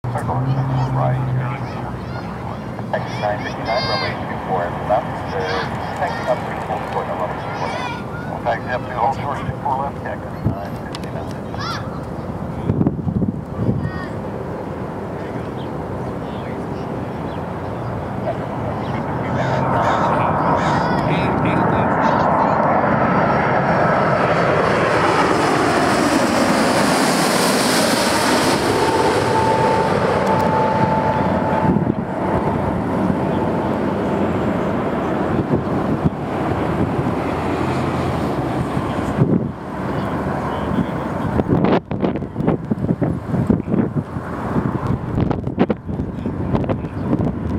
Aeroflot -- Russian Airlines -- Airbus A330-243 [VP-BLX] Landing LAX
Aeroflot - Russian International Airlines Airbus A330-200 Landing at the Los Angeles International Airport.